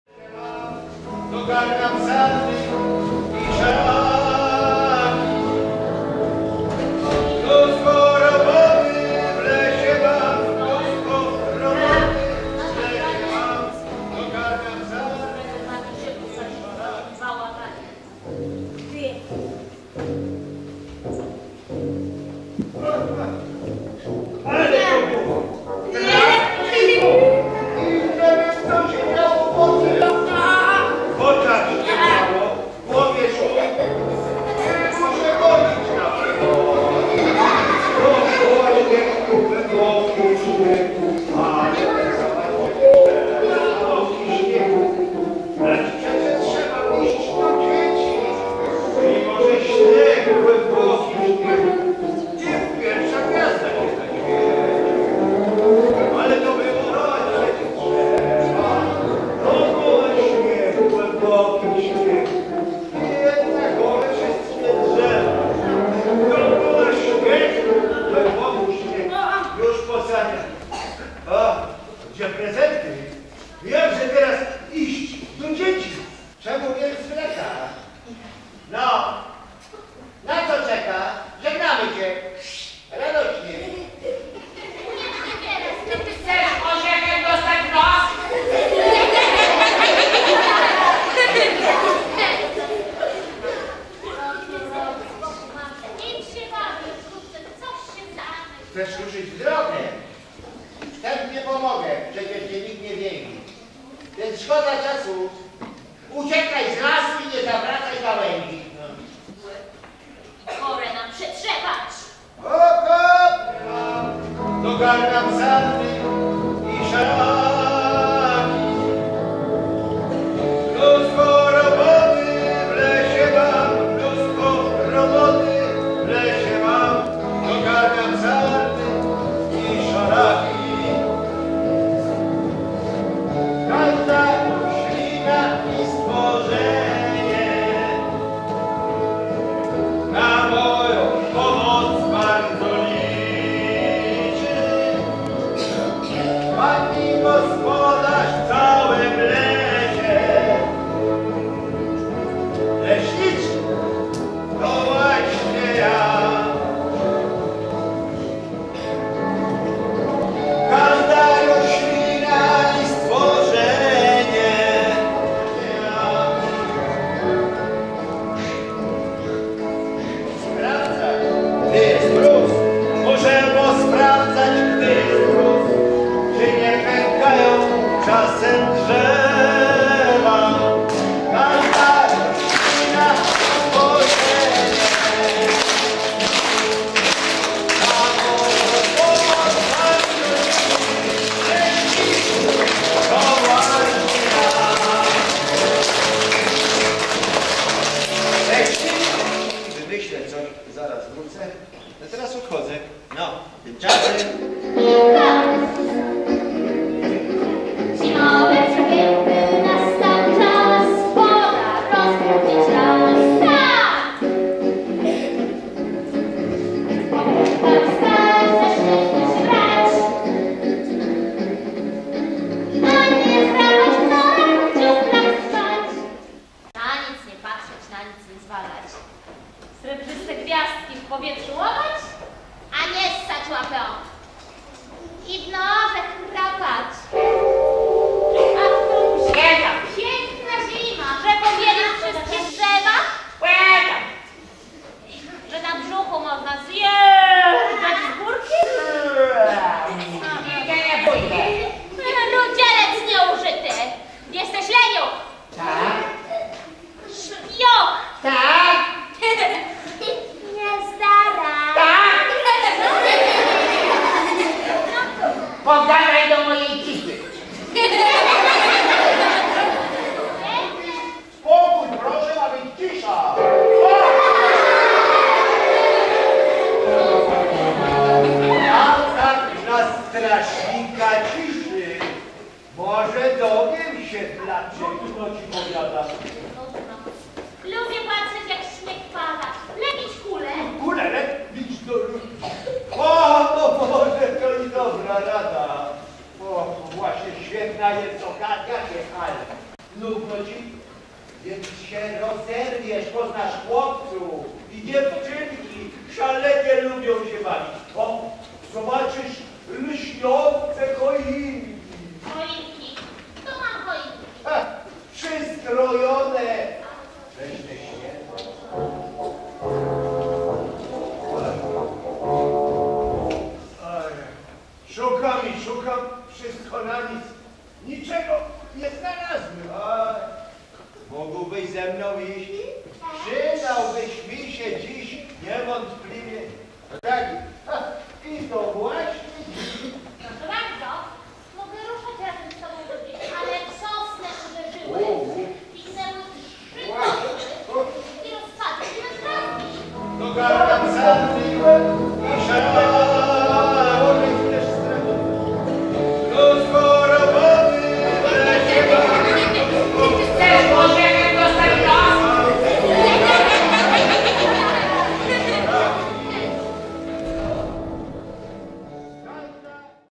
Zimowe niespodzianki
Teatr Lalki i Aktora "Psikus" z Łodzi, prezentuje bajką pt. Zimowe niespodzianki". Przedstawienie odbyło się w Gminnym Ośrodku Kultury w Bedlnie na okoliczność "Mikołajek" - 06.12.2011